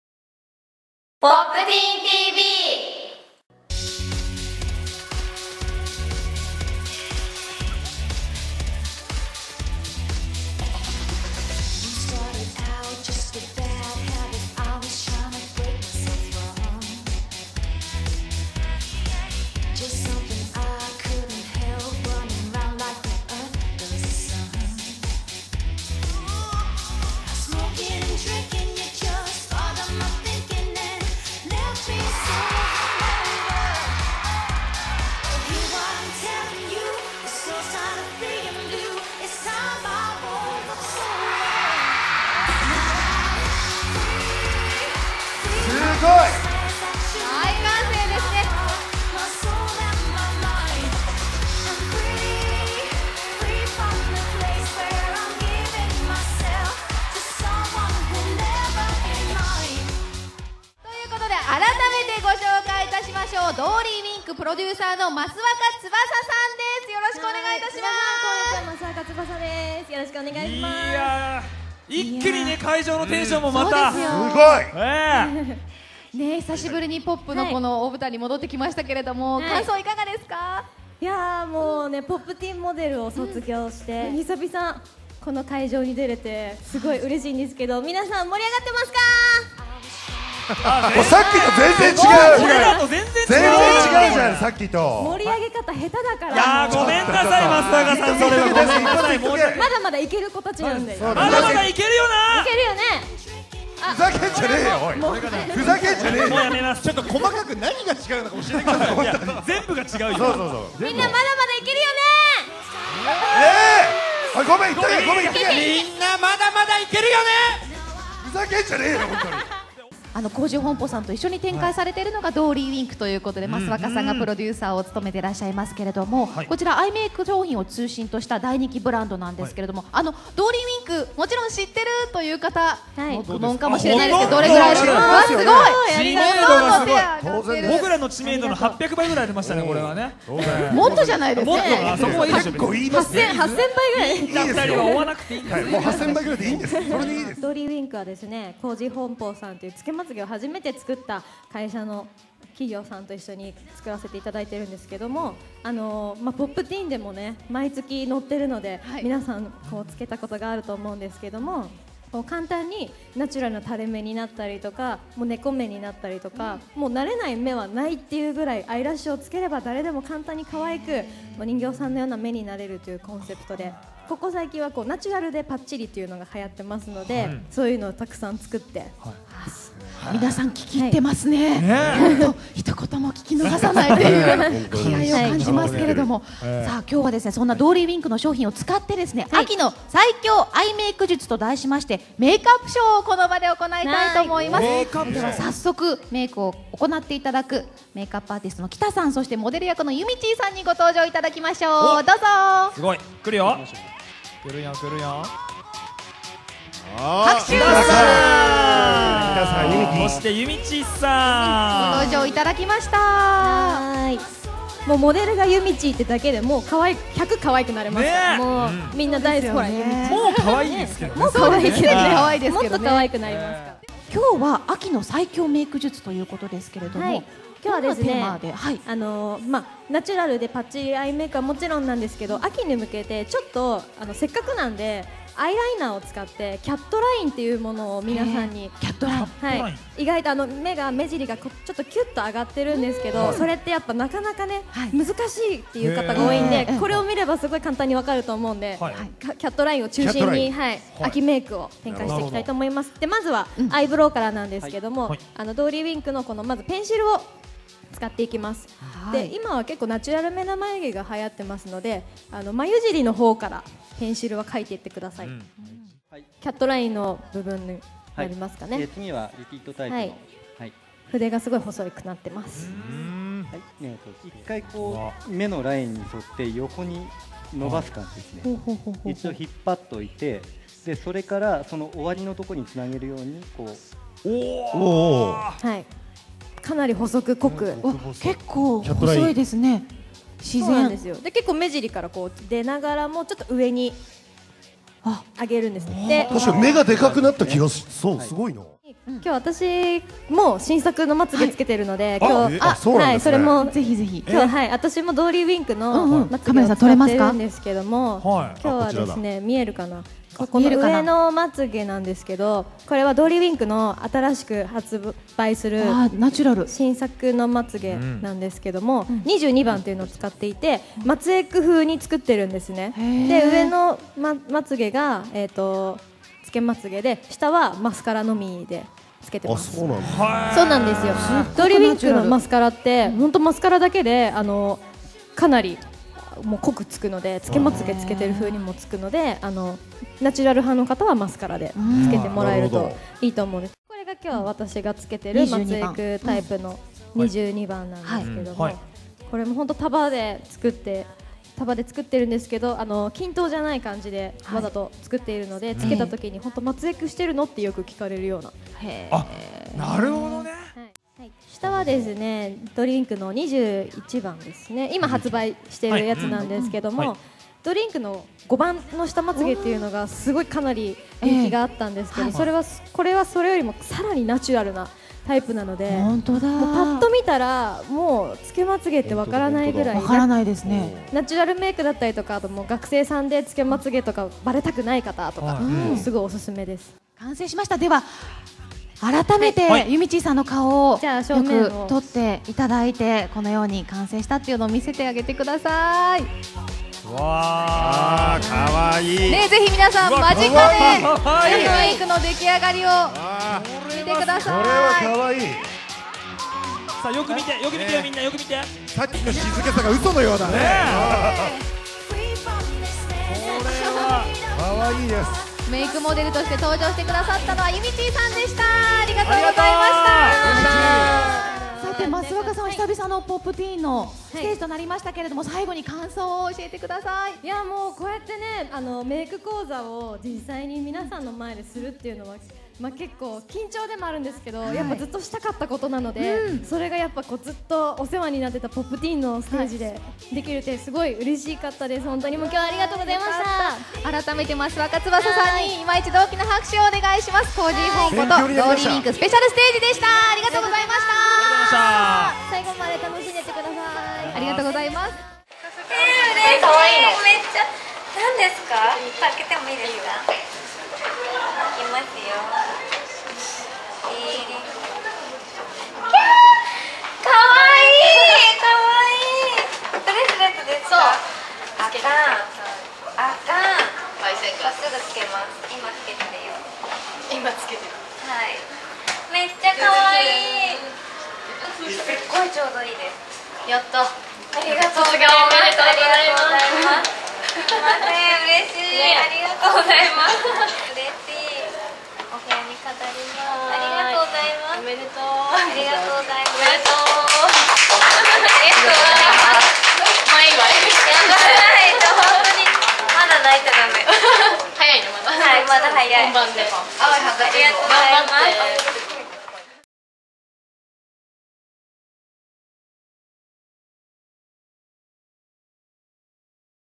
コージー本舗ステージ～Popteen夏の進撃祭2014
OGモデル・つーちゃんこと益若つばさチャンがスペシャルゲストとして登場した、DollyWinkでおなじみコージー本舗ステージを、メイクアップアーティスト・KITAさんがゆみちぃをモデルにしてお送りしたメイクアップショーのごく一部ほか、ダイジェストでお届けします！